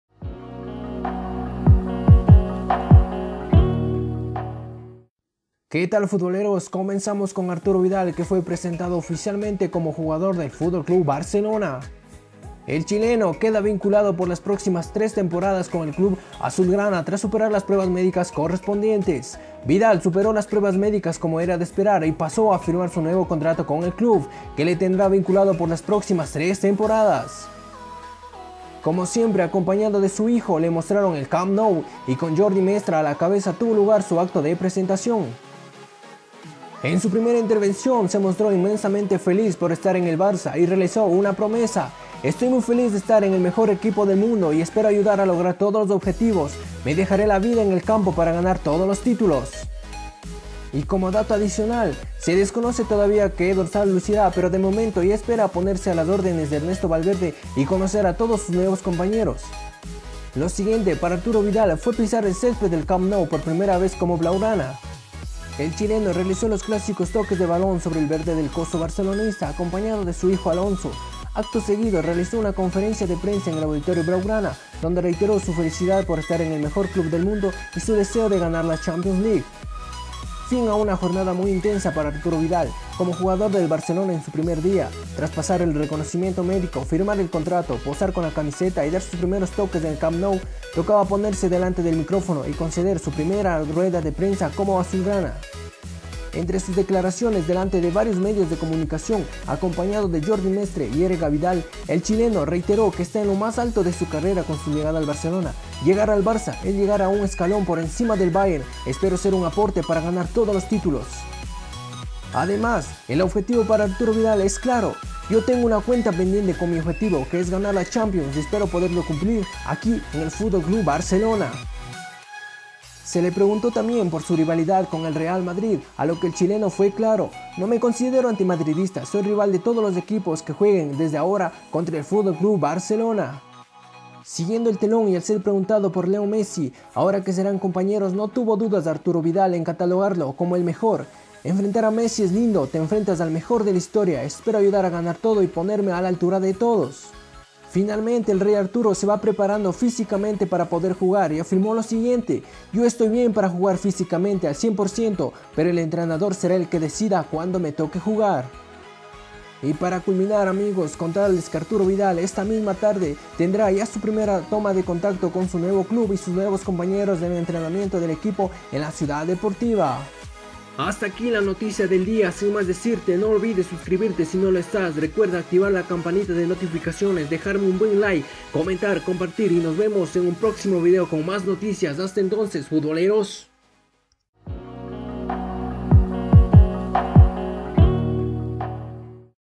Presentación Oficial Arturo Vidal nuevo jugador del FC Barcelona 2018 "antimadridismo, Messi y más"
Presentación Oficial de Arturo Vidal como nuevo jugador del FC Barcelona
el chileno finalmente fue presentado como nuevo jugador blaugrana, firma una contrato de tres temporadas a sus 31 años llega al Camp Nou para reforzar la medular del medio campo Culé que se ha vuelto vulnerable tras la salida de Iniesta y Paulinho.
Vidal llega procedente del Bayern Múnich con un propósito el conseguir la tan anhelada Champions título que se le a resistido ya en varias ocasiones.
Vidal tras ser presentado mostró su felicidad de jugar con Messi, Suárez y compañía, esperando conseguir la mayoría de títulos en los años de contrato que tiene. tras ser presentado piso el Camp Nou por primera vez como Blaugrana hizo los respectivos toques y más tarde conoció al técnico Valverde y a sus nuevos compañeros de la plantilla Culé así Vidal es el tercer Chileno en vestir los colores del FC BARCELONA tras " Alexis Sánchez y Claudio Bravo"
su debut se podría dar este domingo frente al Sevilla por la Super Copa de España.
Activar la campanita de Notificaciones para que te llegue cada que se suba un vídeo.